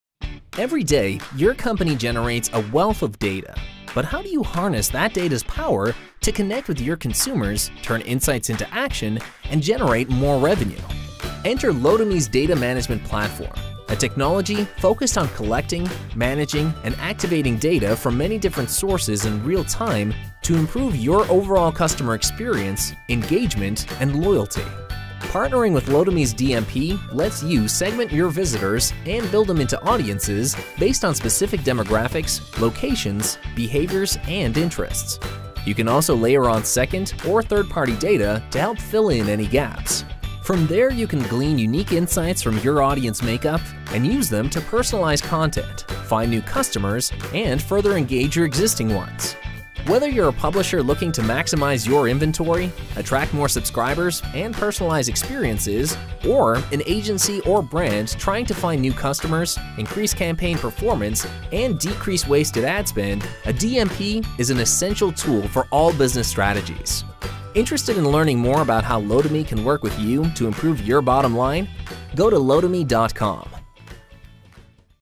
What is a data management platform (DMP)?
Your company generates a wealth of data. But how do you use that data to collect more insights on your consumers and increase revenue? Learn how a DMP helps.